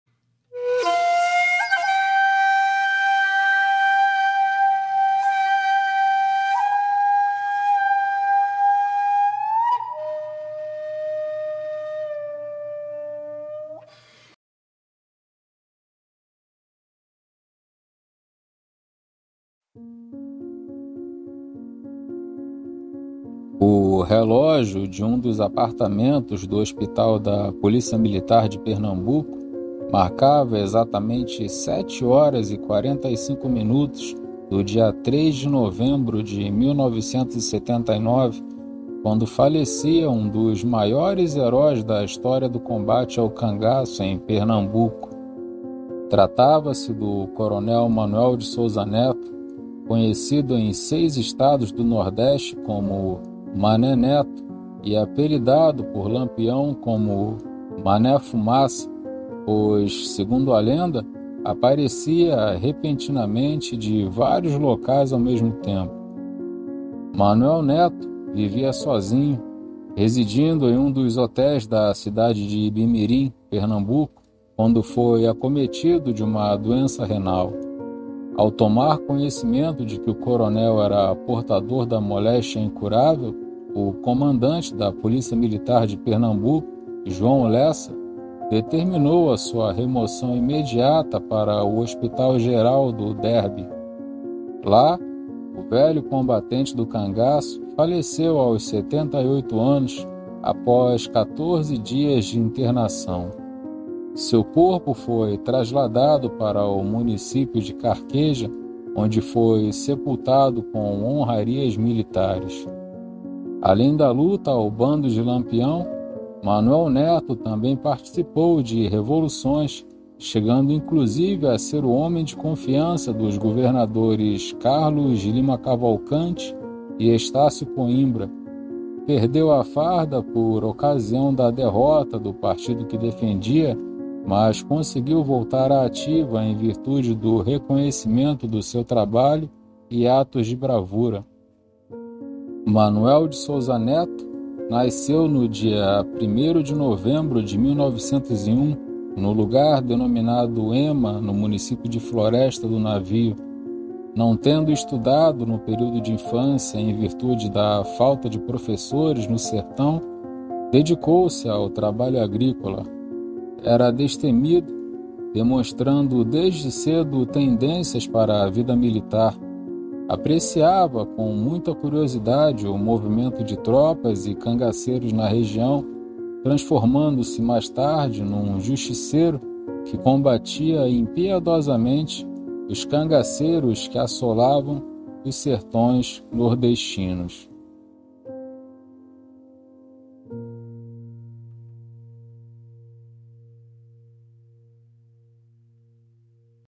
Cangaço: Mané Neto Morre Solitário
"O relógio de um dos apartamentos do Hospital da Policia Militar de Pernambuco,marcava exatamente sete horas e quarenta e cinco minutos do dia 3 de novembro (de 1979), quando falecia um dos maiores heróis da história do combate ao cangaço em Pernambuco. Tratava-se do coronel Manoel de Souza Neto, conhecido em seis estados do nordeste como "Mané Neto" e apelidado por Lampião como "Mané fumaça"..."

Referências:
."Diário de Pernambuco, edição de 11 de Novembro de 1979"
."Morreu solitário o homem que Lampião mais temia", por Marco Túlio
.Blog "Lampião Aceso"

Imagens:
.Lampião Aceso